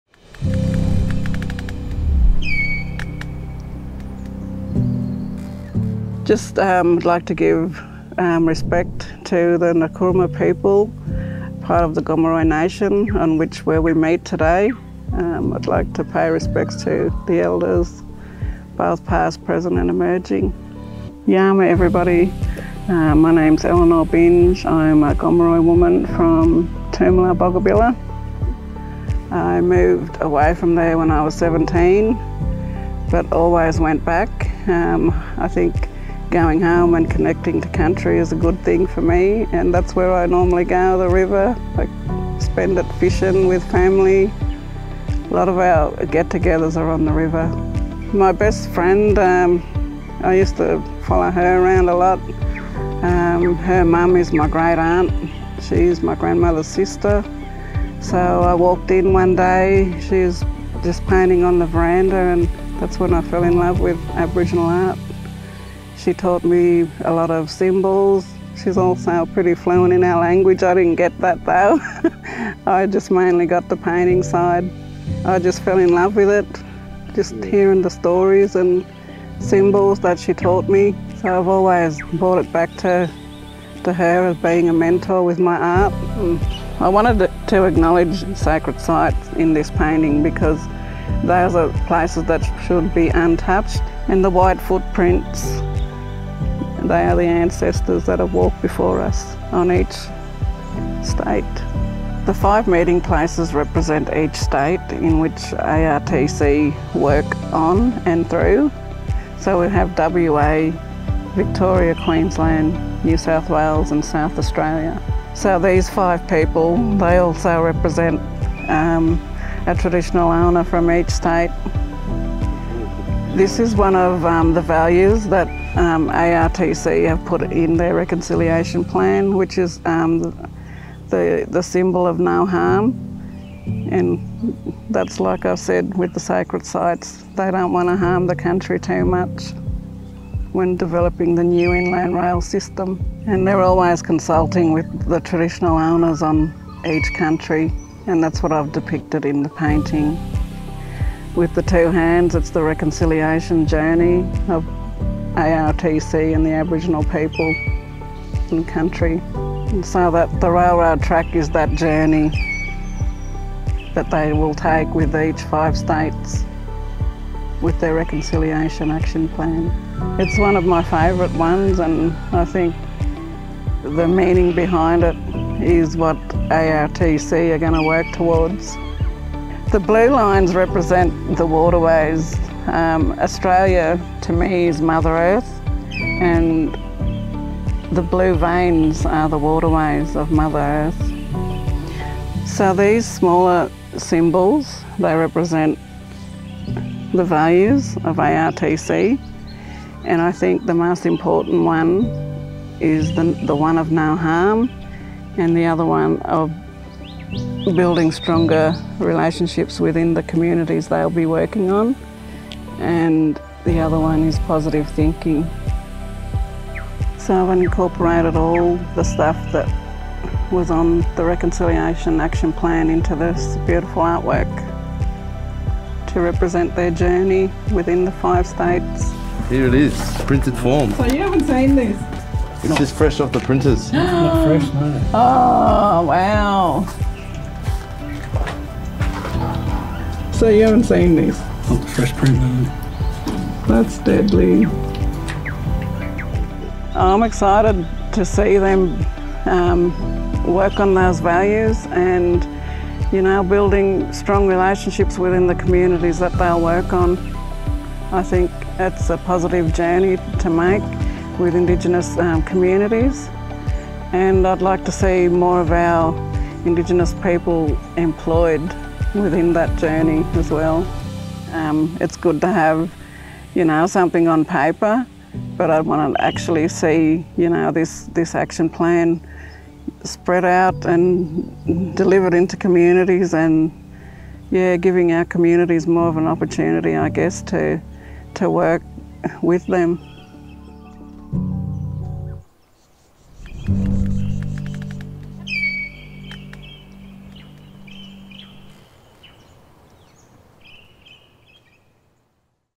Elenore Binge teaches us the symbols in her artwork Journey: the heart of Reconciliation at ARTC.
At the heart of our reconciliation action plan (RAP) is Journey, created by artist Elenore Binge.
Produced by Kieran Satour, the GARUWA team have beautifully captured Elenore's story and the lands of the Kamilaroi People in Inverell.
Elenore teaches us about the symbols and meaning of her work and its connection to Reconciliation at ARTC.